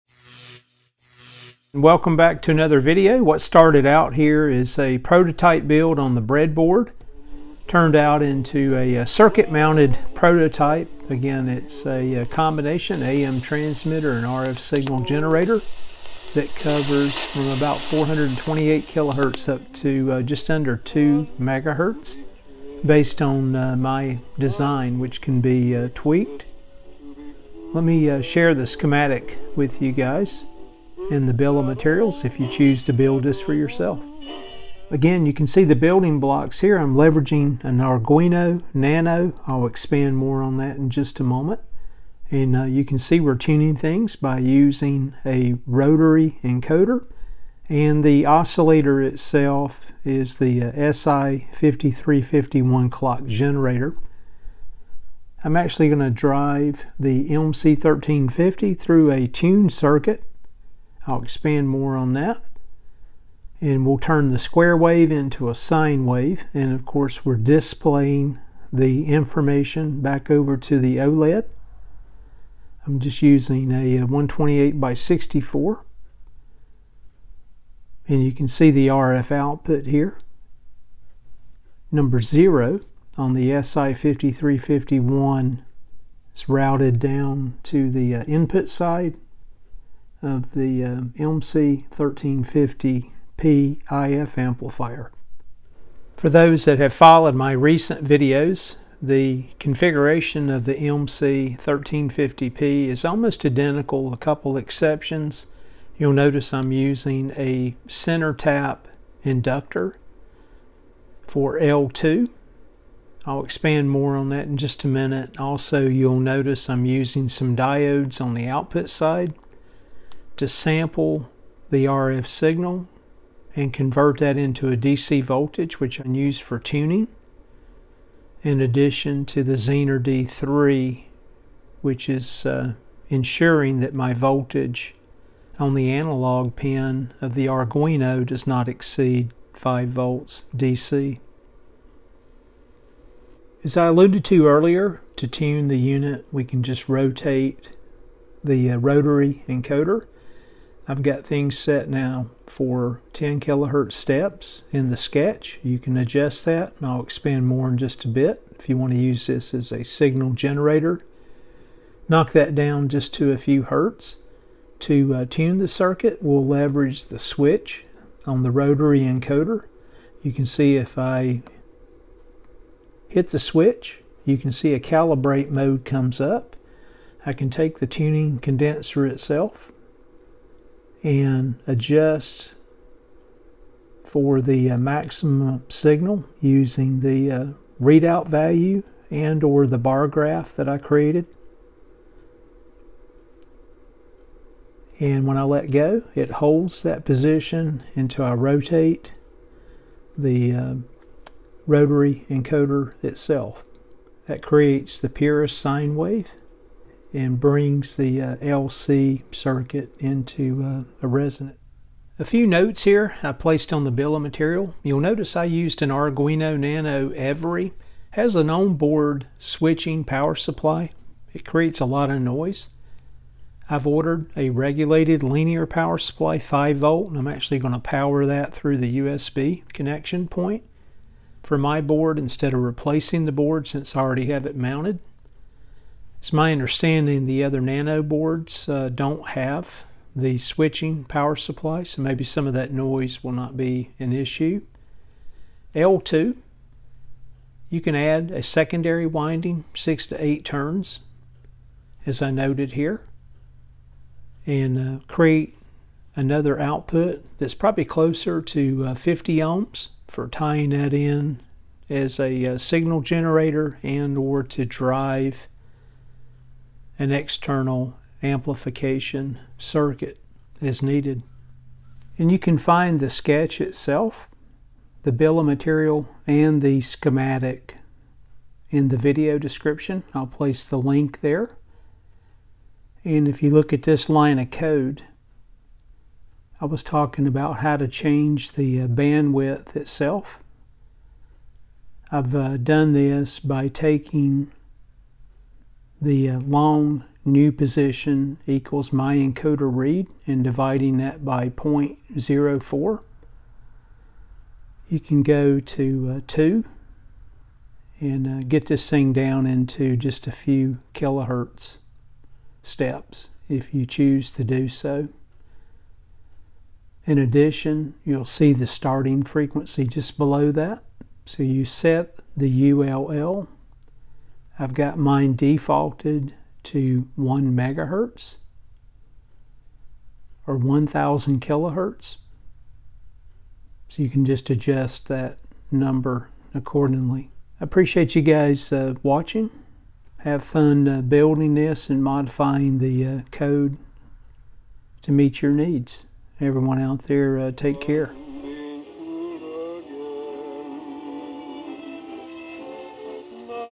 Build an AM Transmitter / RF Signal Generator w/ Arduino
A close proximity AM Transmitter or RF Signal Generator w/ a few simple modifications using a Arduino Nano, Si5351 Clock Generator, OLED 128x64, MC1350P IF Amplifier and a few other... The LC circuit by design covers 428 kHz to 1.9 MHz, but can be modified as needed.